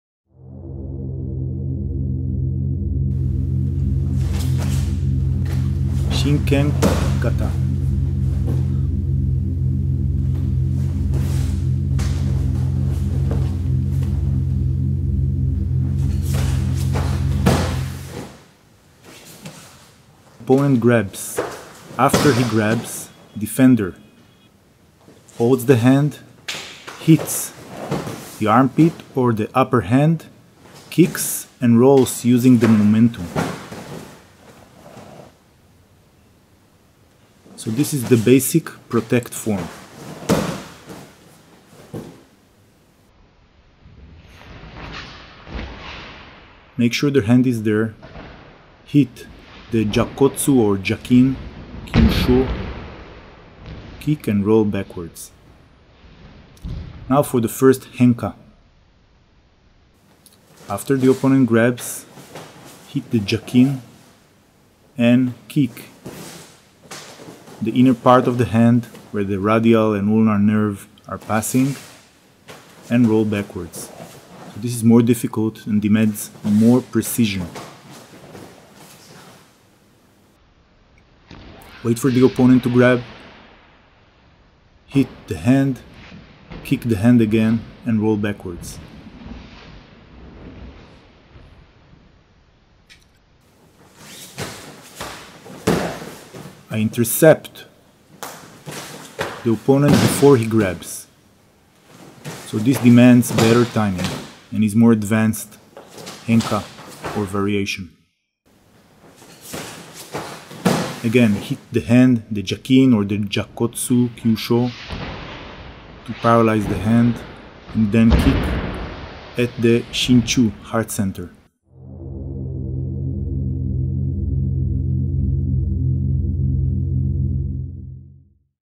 Shinken kata, Chi no kata, Shinden Fudō Ryū Dakentaijutsu for the AKBAN wiki
Two veterans from the AKBAN academy document the basic Shinken Kata and variations